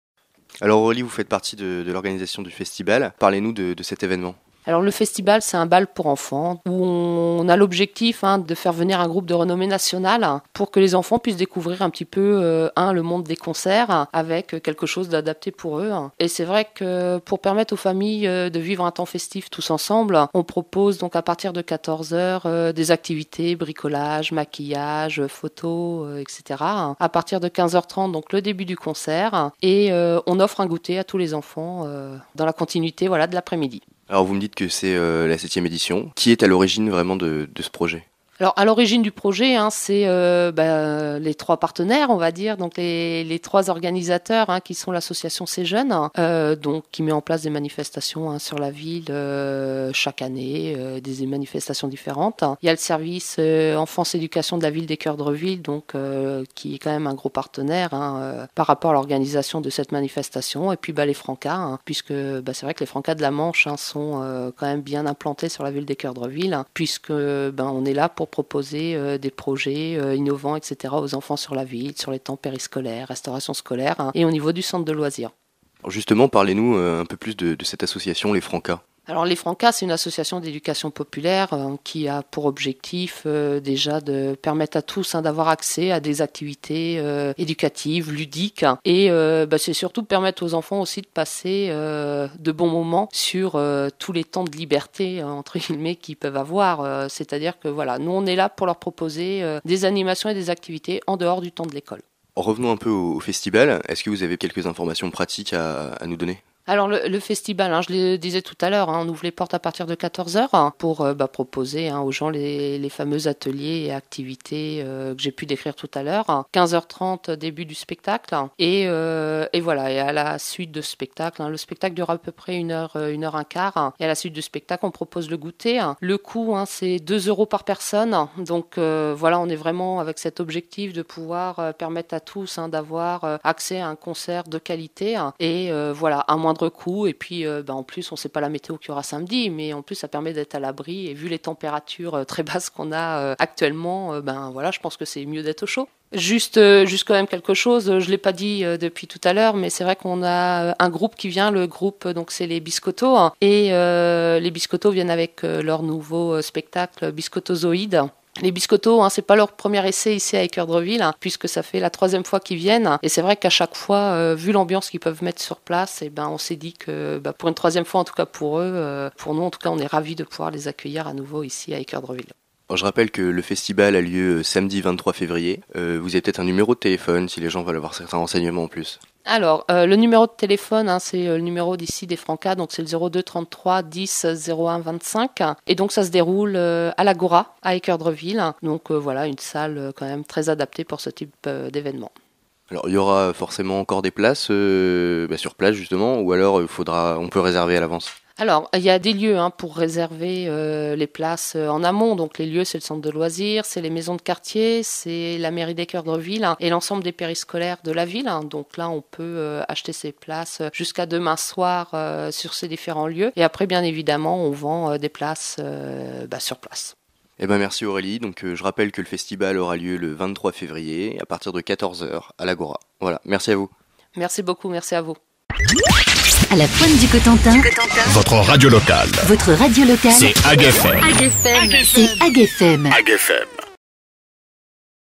Radio HAG' FM - Le Festibal des enfants
Samedi 23 février a lieu la 7ème édition du Festibal des enfants.
Aurélie, organisatrice de l'évènement, répondait à nos questions.
Des propos recueillis par Romain pour HAG' FM.